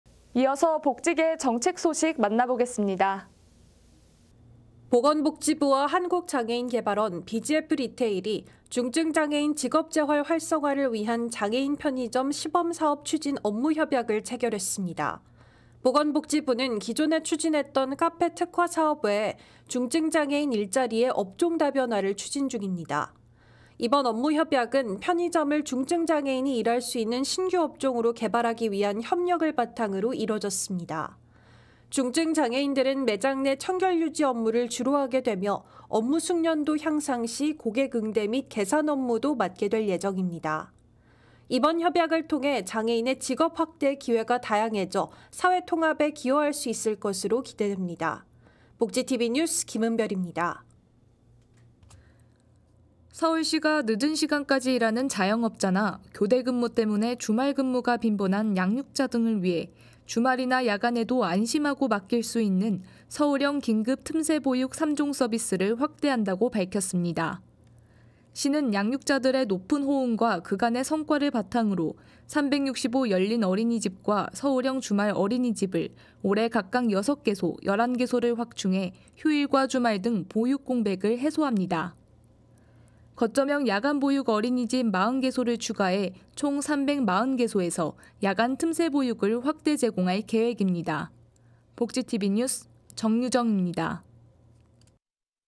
복지TV뉴스24_정책24 (2024.03.20)
정책 1)보건복지부 중증장애인 일자리 업종 다변화 추진
정책 2) 서울시, 야간,주말 근로 양육자 위한 긴급틈새 보육 서비스  강화